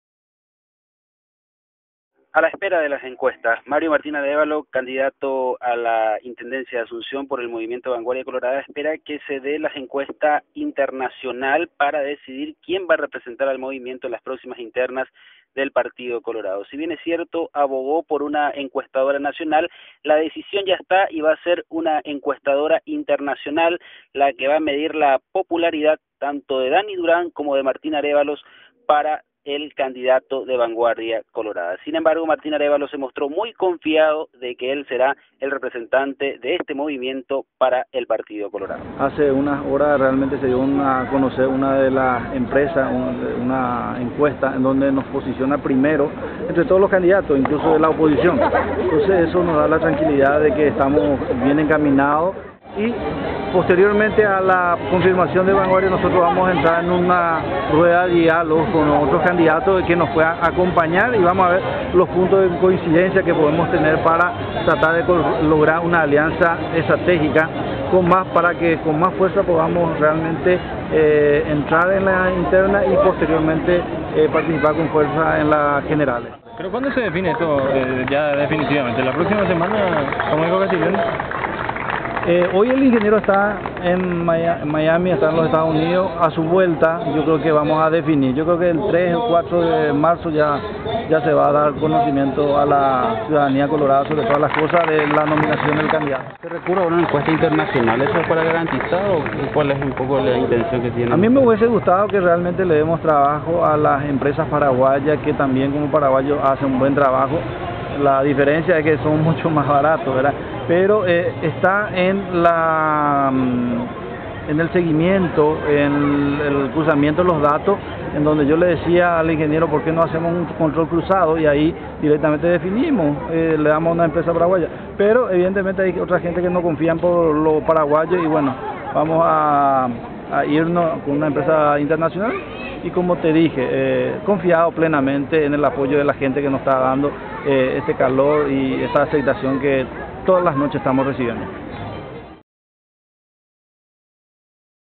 Entrevista con Martin Arévalo
El pre-candidato por Vanguardia Colorada, Martin Arévalo, brindó declaraciones entre dos tiempos de un acto multidinario realizado en Loma Pyta.